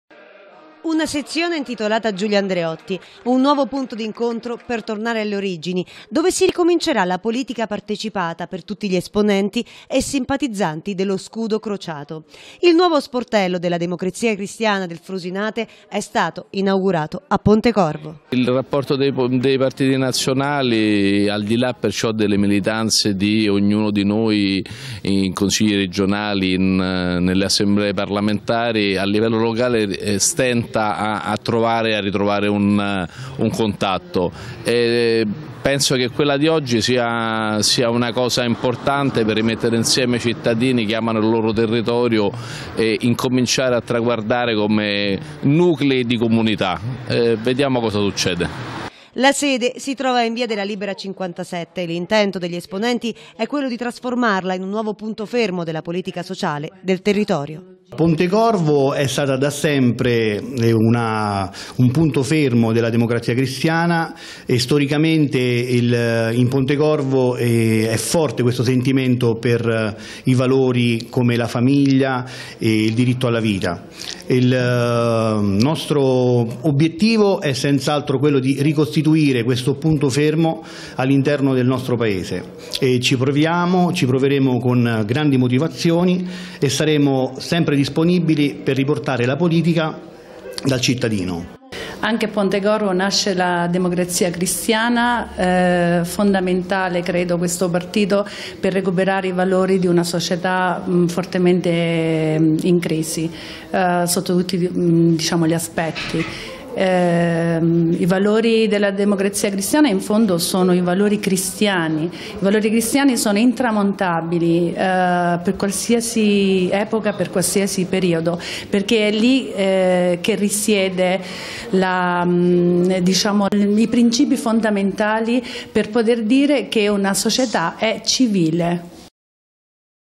PONTECORVO: Inaugurazione sezione DC del 09-04-2017
Un nuovo punto di incontro, una sezione per far ripartire la politica da parte di tutti gli esponenti della Democrazia Cristiana del frusinate. La sede è stata inaugurata a Pontecorvo.